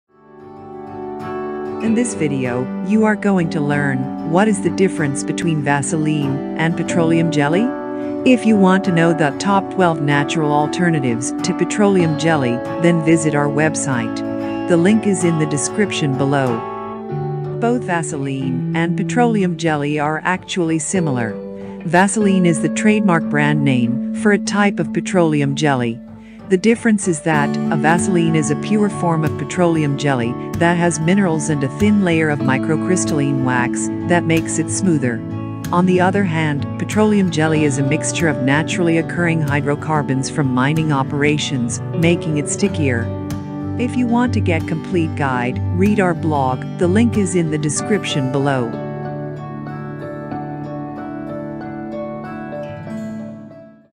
Vaseline And Petroleum Jelly! What's The Difference?
Read Our Blog for Complete Guide!

If you have sensitive skin and have been advised by your doctor to avoid petroleum jelly, then say goodbye to petroleum jelly.
You may be wondering what are some natural alternatives to petroleum jelly that won’t cause irritation or worsen your skin condition.
Luckily, there are plenty of other options to help you keep your skin soft and moisturized without putting any chemicals on your body.
This article will give you some of the best options to try so that you can maintain soft, supple skin without the worry of causing yourself further harm.
From coconut oil to shea butter, these alternatives will help you achieve healthy and radiant skin without the use of petroleum jelly or other harmful chemicals that can be found in many store-bought products.
The great thing about these natural alternatives is that they are all available at your local pharmacy or supermarket, making them easy and affordable!
Want to learn more? Keep scrolling!
What is Petroleum Jelly?
Petroleum jelly is a semi-solid mixture of hydrocarbons obtained from petroleum. It was discovered in 1859 by Robert Augustus Chesebrough.
It was used in medicine as a treatment for burns and wounds. Since then, it has been widely used for numerous purposes including its use as an ingredient in cosmetics.
Some of these are commonly found in crude oil while others are not normally found. Petroleum jelly is also known as white petrolatum or paraffin wax.
What Is The Difference Between Vaseline And Petroleum Jelly?
Vaseline is the trademarked brand name for a type of petroleum jelly. The difference is that a Vaseline is a pure form of petroleum jelly that has minerals and a thin layer of microcrystalline wax, making it smoother. Petroleum jelly is a mixture of naturally-occurring hydrocarbons from mining operations, making it stickier.